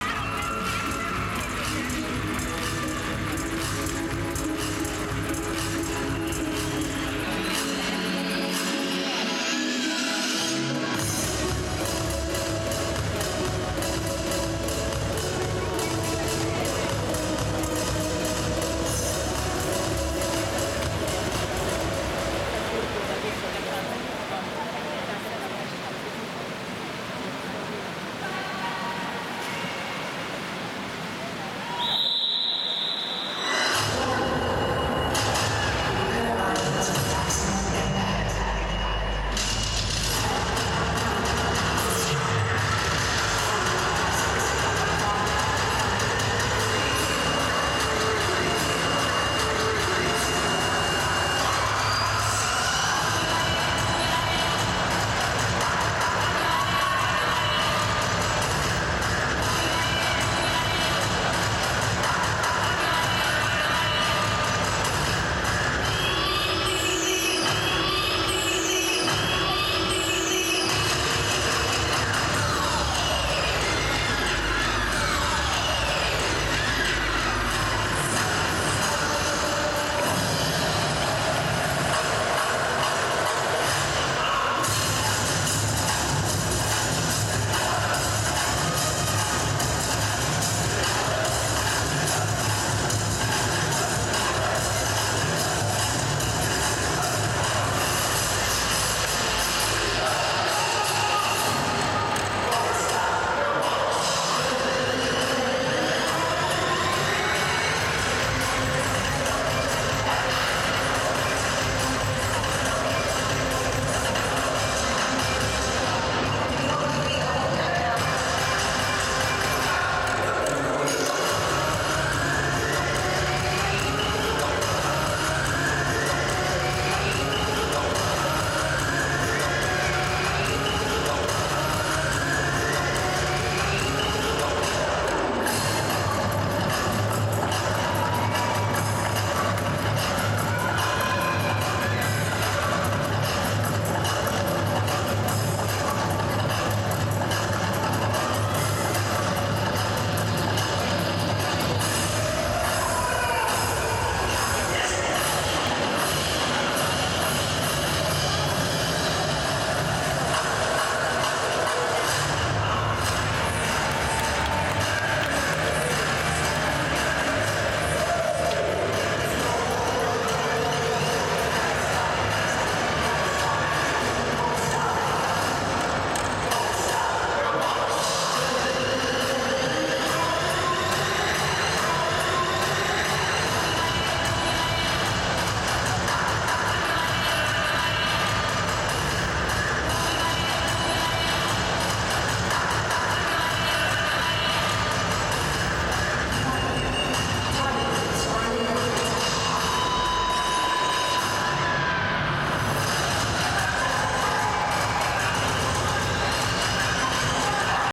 Artistic Swimming Team Acrobatic 2024-Australia
World Aquatics Artistic Swimming World Cup 2024 Super Final,
Budapest, Hungary
Team Acrobatic

Carolyn Rayna Buckle, Raphaelle Gauthier, Margo Joseph-Kuo, Zoe Poulis
Georgia Courage-Gardiner, Kiera Gazzard, Putu Anastasia Kusmawan, Milena Waldmann
Australia